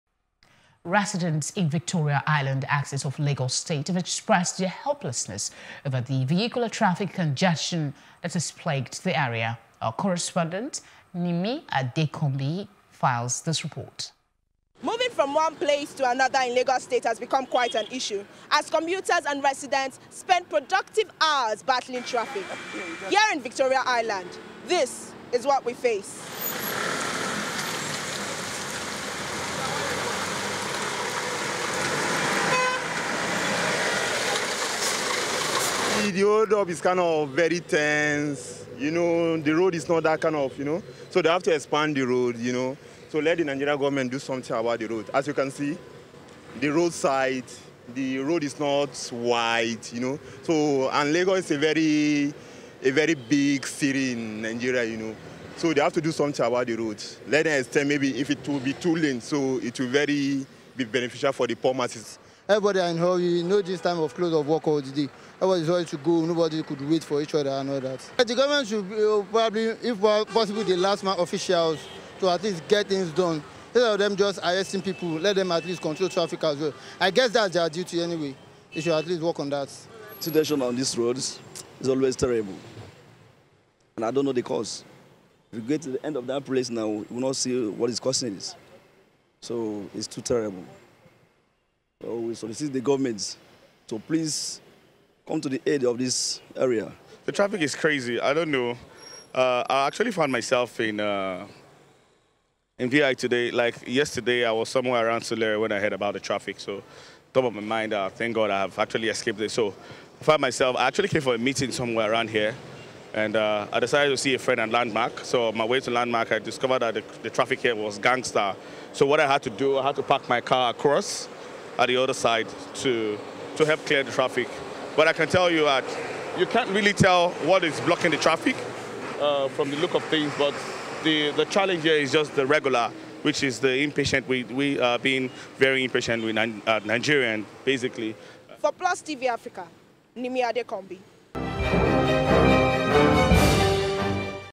Victoria Island Residents Seek Govt. Intervention For Traffic Congestion
Residents in Victoria Island axis of Lagos state have expressed their helplessness over the vehicular traffic congestion that has plagued the area.

Our correspondent, Nimi Adekanmbi files this report